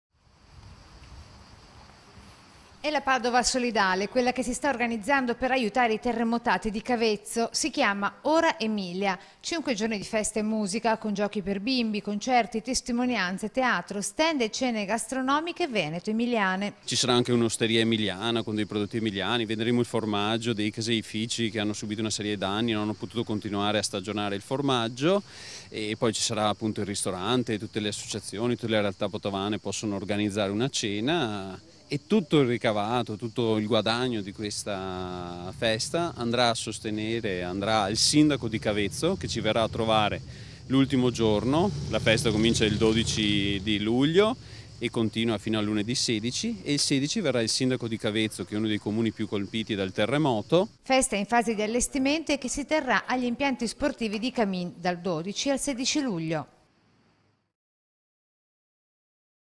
A CAMIN FESTA DI RACCOLTA FONDI PER I TERREMOTATI DI CAVEZZO
5 serate di festa per raccogliere fondi per l'Emilia.
Festa di solidarietà per i terremotati del sisma di Cavezzo. Il servizio di Rete Veneta TG Padova